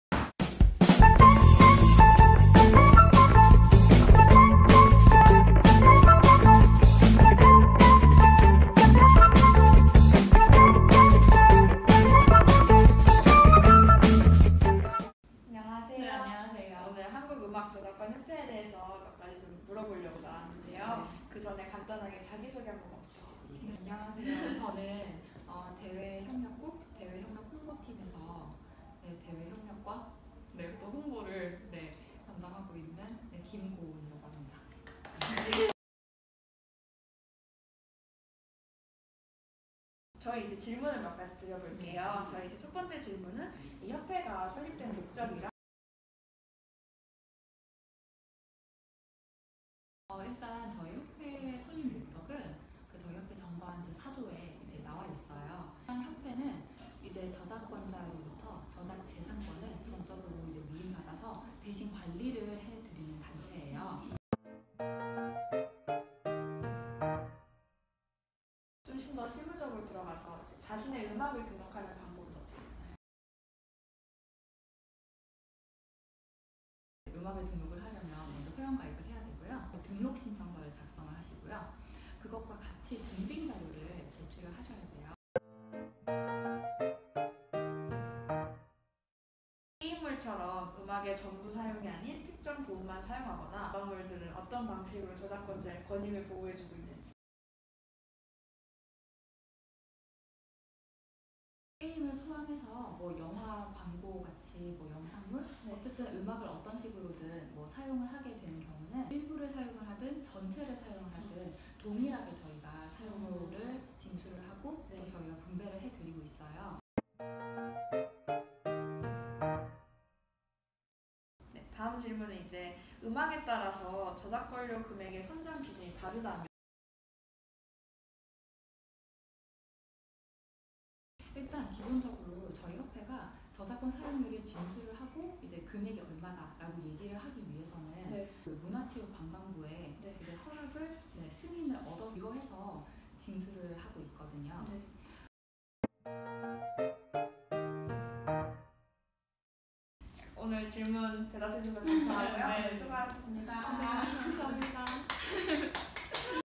반듯ⓒ 저작권 서포터즈 10월 2차 영상5팀 ⓒ씨쓰루
[한국음악저작권협회 소개 및 업무 인터뷰 영상]

알짜배기만 담아온
한국음악저작권협회
인터뷰 지금 시작합니다!

올바른 저작물 이용으로
반듯한 저작권 문화를 만들어요!